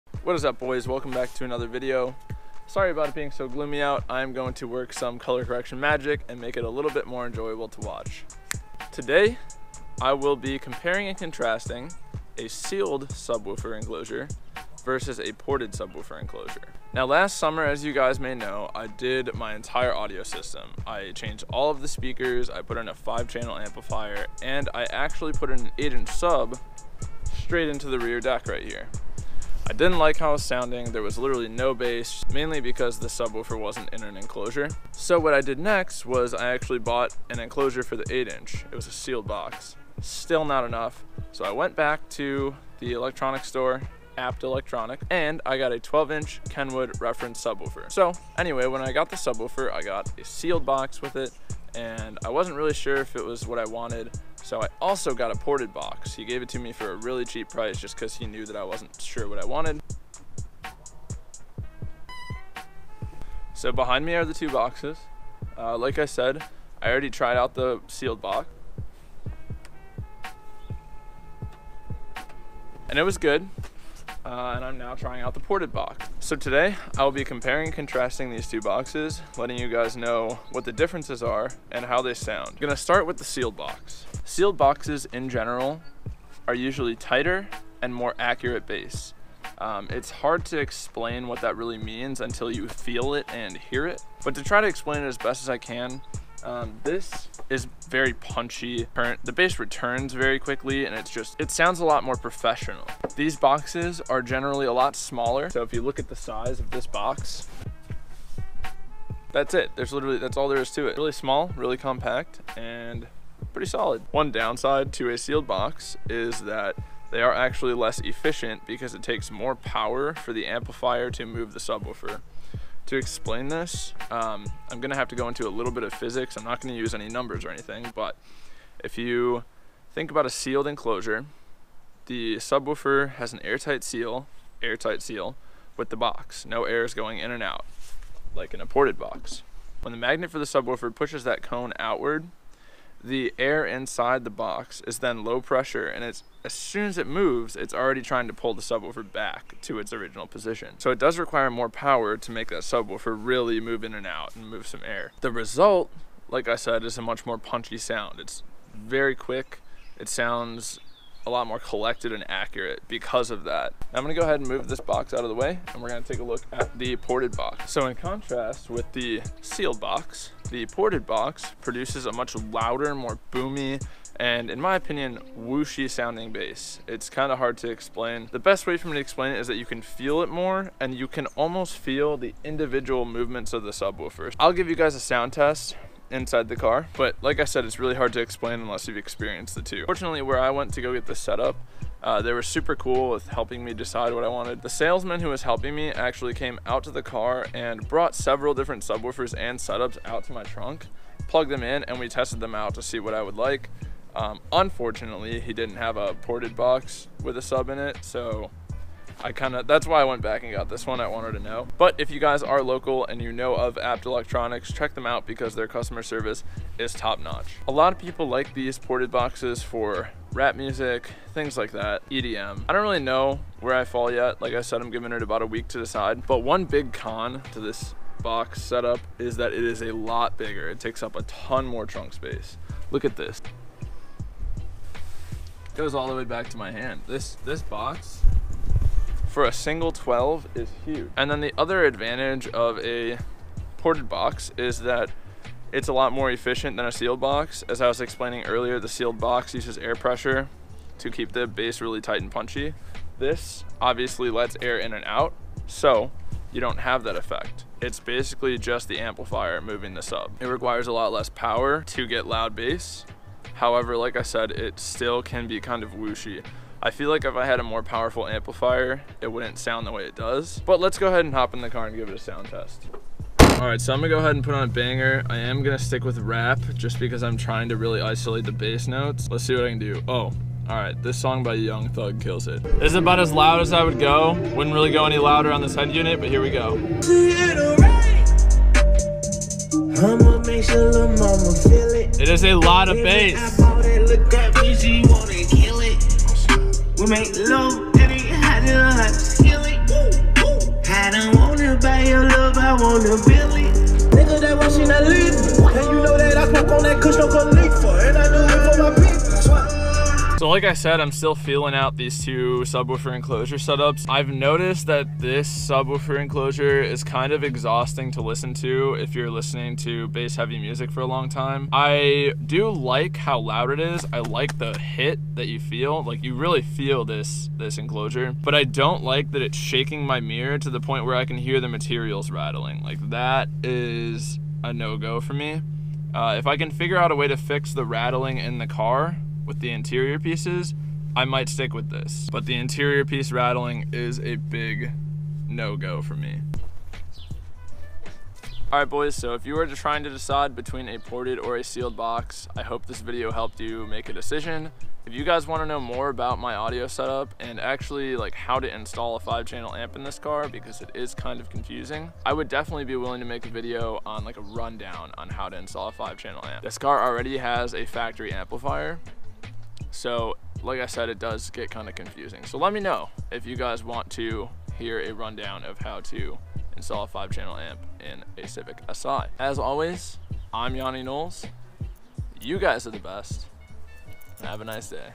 Subwoofer Enclosure: Sealed VS. Ported
Today I wanted to compare my sealed enclosure to my ported enclosure. Hopefully this helps you guys make a decision.